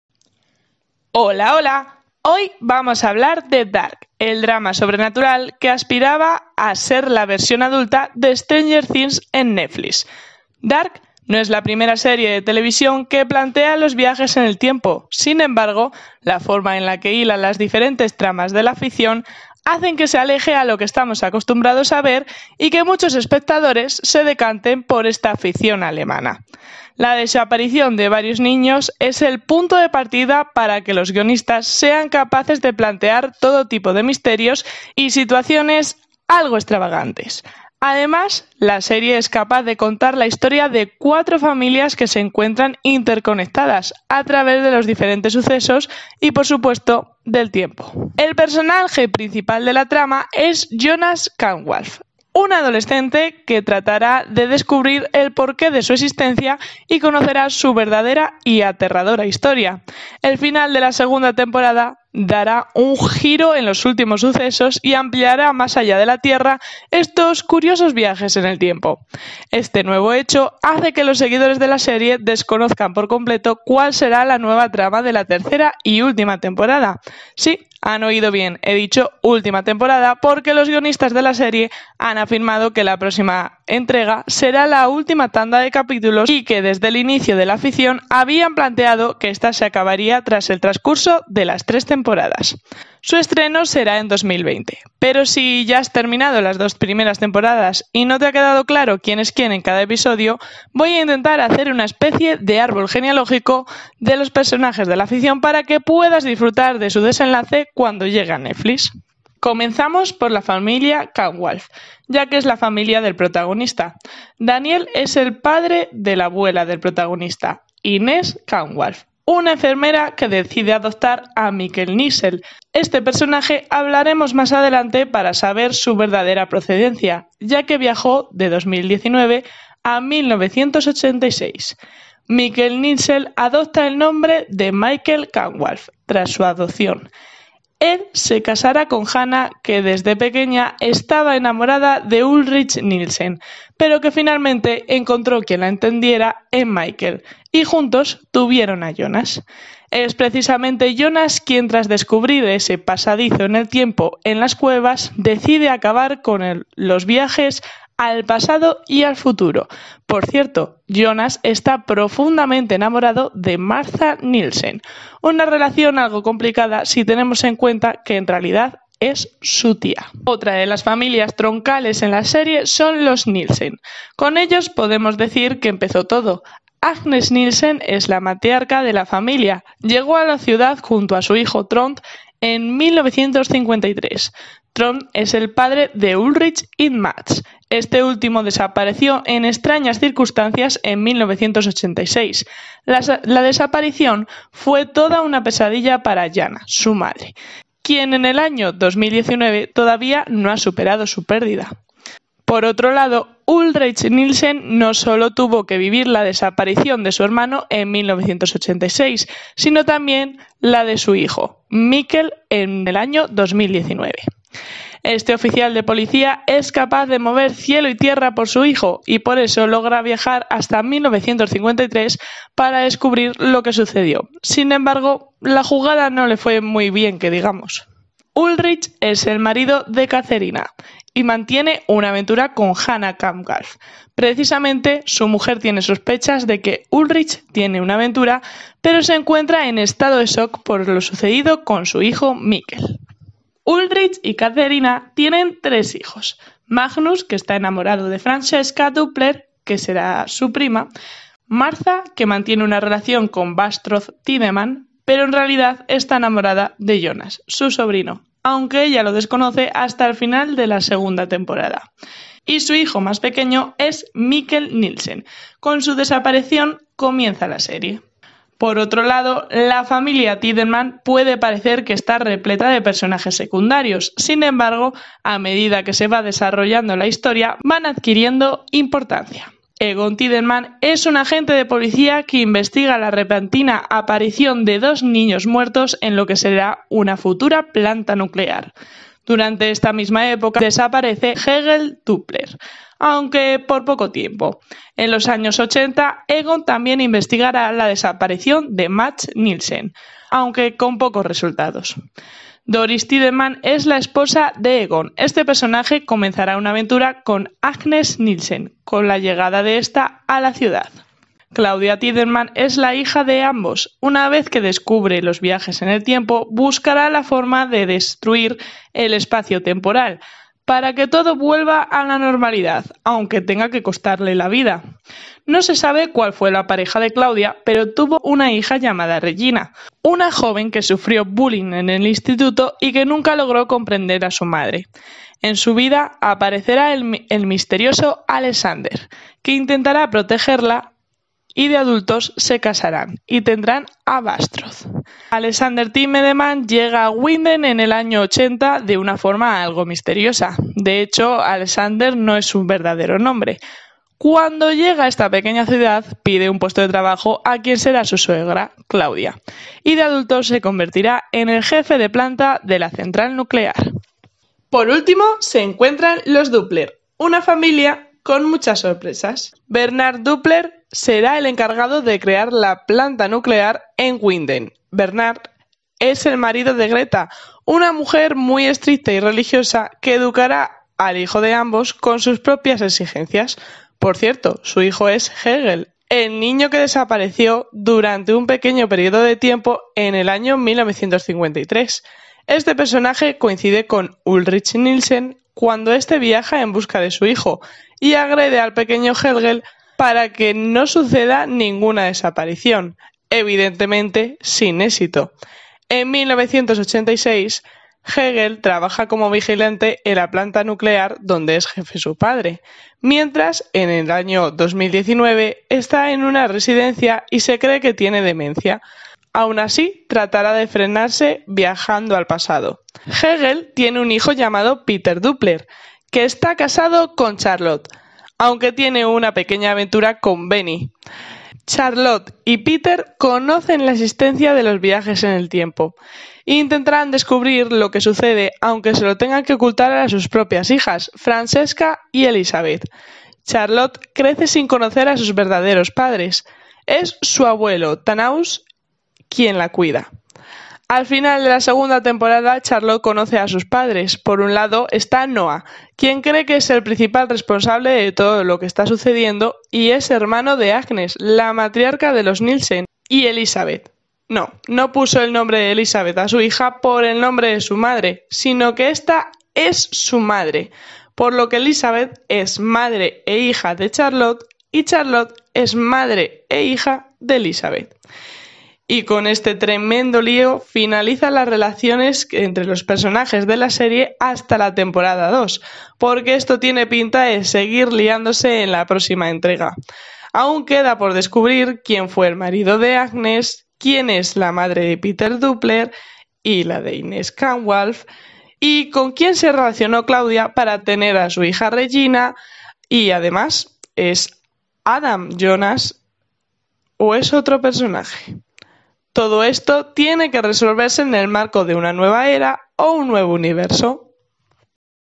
Dark (Netflix): ¿Quién es quién?
Este vídeo contiene los SPOILERS NECESARIOS PARA CONOCER CON EXACTITUD QUIÉN ES QUIÉN en la serie de producción alemana.
El árbol genealógico está actualizado hasta la segunda temporada. Los guionistas de la ficción han asegurado que la tercera entrega será la última tanda de capítulos de la familia Kahnwald, Nielsen, Tiedemann, Doppler y el resto de habitantes de Winden.